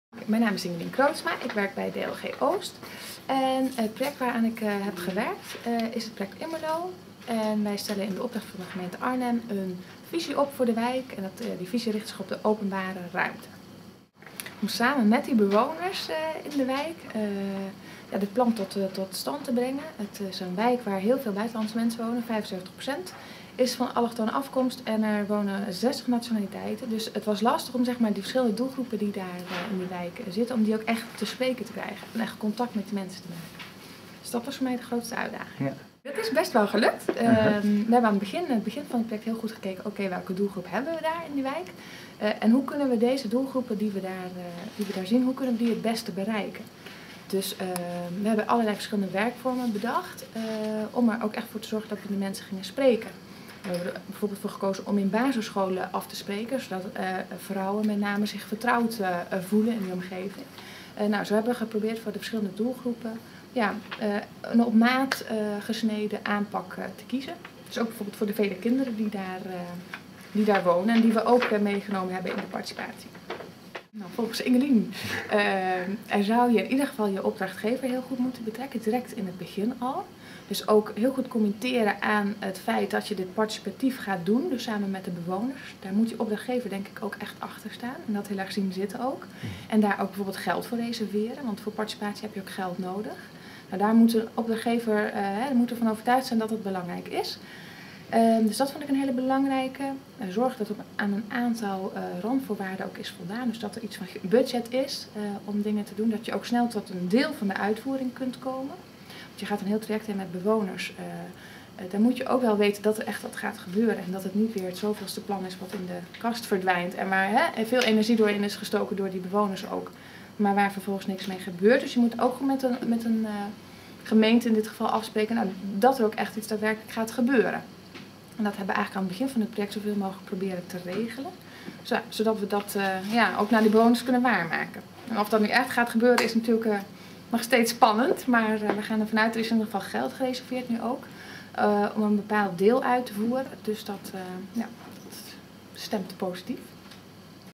Publieksparticipatie in Arnhem
Interview met Ingelien Kroodsma (Dienst Landelijk Gebied) over haar aanpak bij het maken van een inrichtingsplan in de wijk Immerloo in Arnhem. Bewoners en scholieren zijn intensief betrokken bij het opstellen van een plan voor hun buurt.